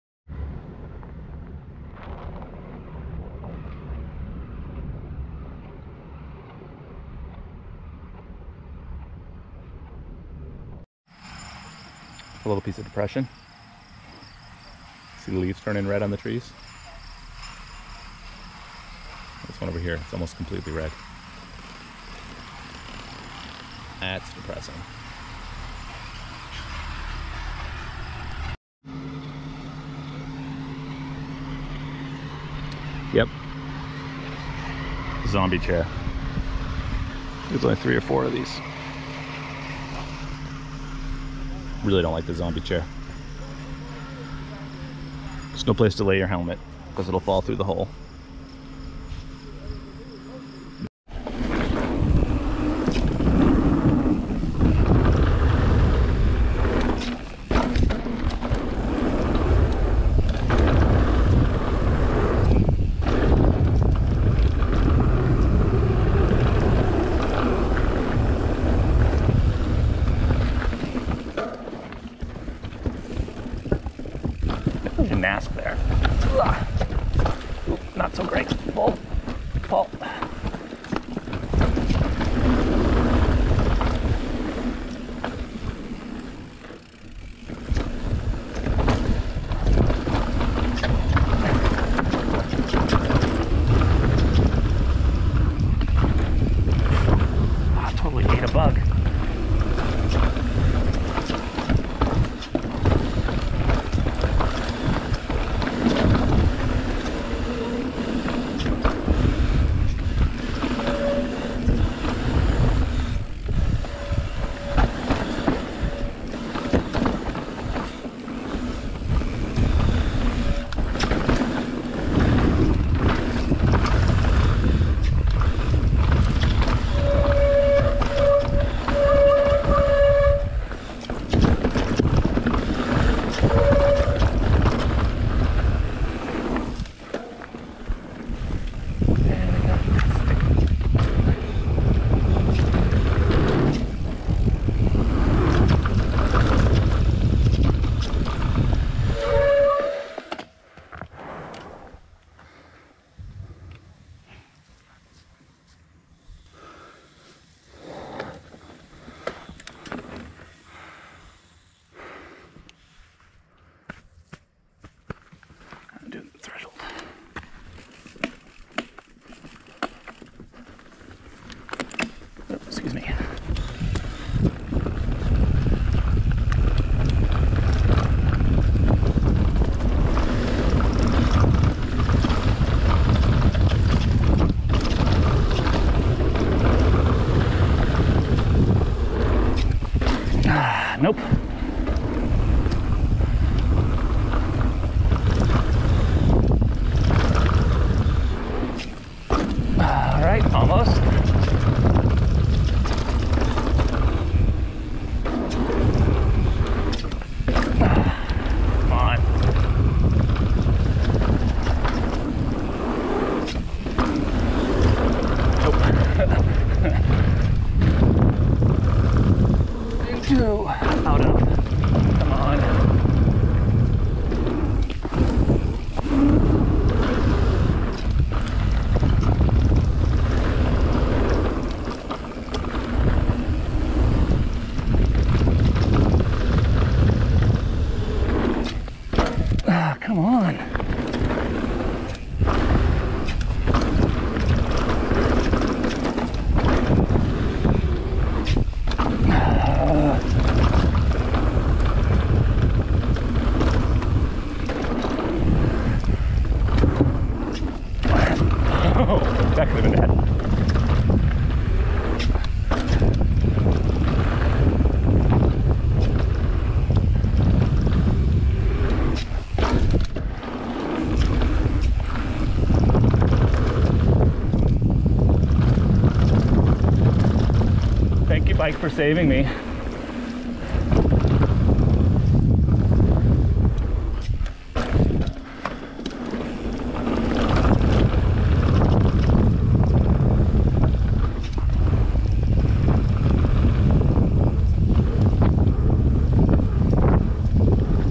Highland Bike Park, views from the lift and Hellion/Bone Saw/Hellion run
Just a nice view from the lift, a few signs of fall coming, and the ride on one of the zombie chairs. Remnants from the original lifts that they used exclusively up until this year...I mean besides the 4 or 5 that are still here. Look at the condition and think "they kept the best ones" to realize how terrible they all were last year.

I did one of the jumps on Hellion, which I normally avoid, but also did it in the same points of view as the earlier released videos. There is one clip out in there due to. language. :-)